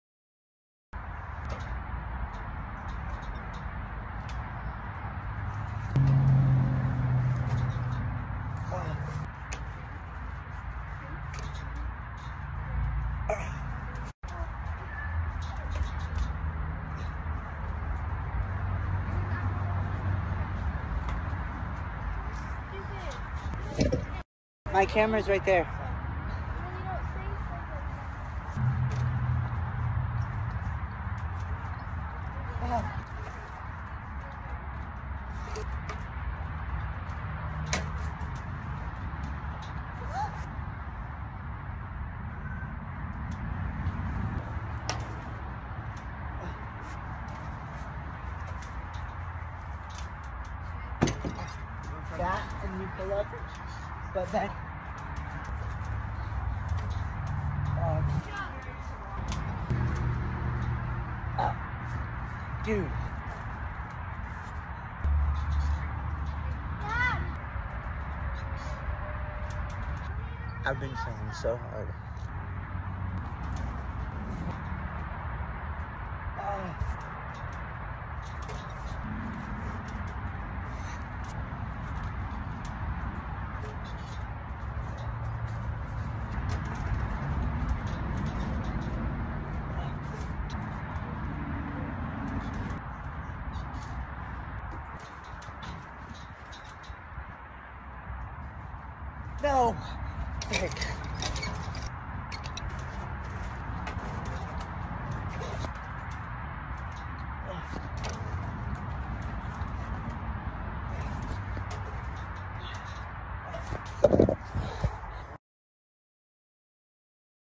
Practicing for a Muscle up!
Story is: i was at this place with a “pull up bar” so i trued a muscle up. Then i did one. My first one. And my form was a little bad. But. Not for my first one. I did great. Then i realized i wasn’t filming. So i got out my phone. And here is the video.